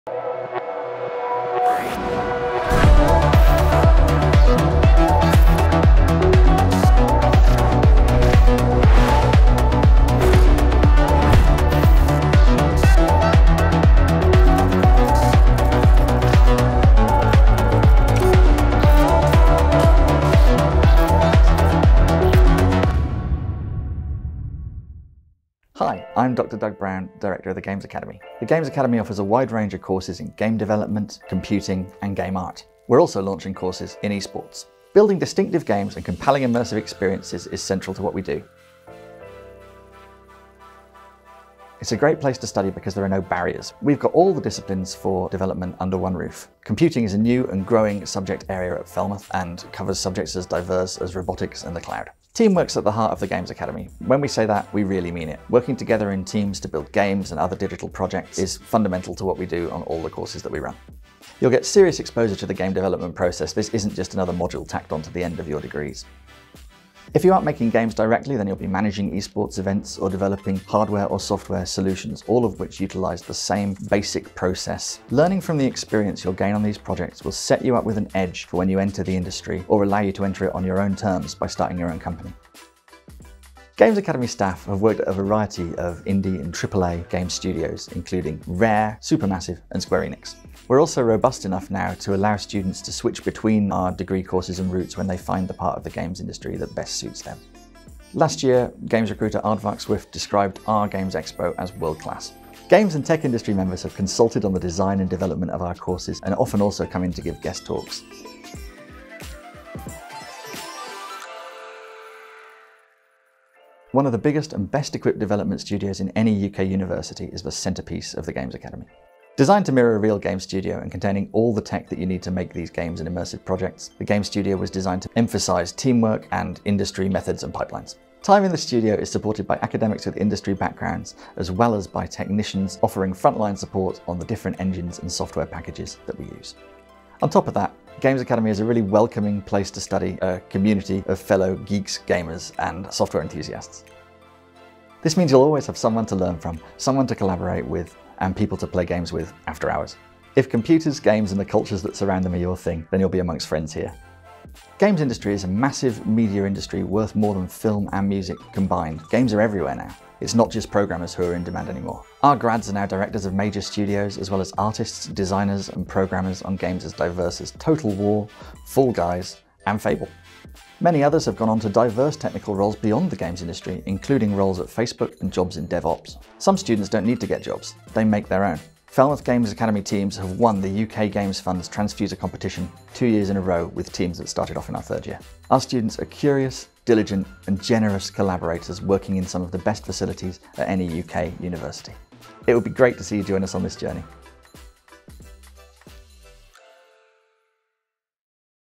Games Academy | Falmouth University
Our Games Academy has been recognised on an international scale for its innovative approach. We’re now ranked in The Rookies Best Game Design & Development Schools in the World 2020.

Head over to our website to find out more about the Games Academy at Falmouth University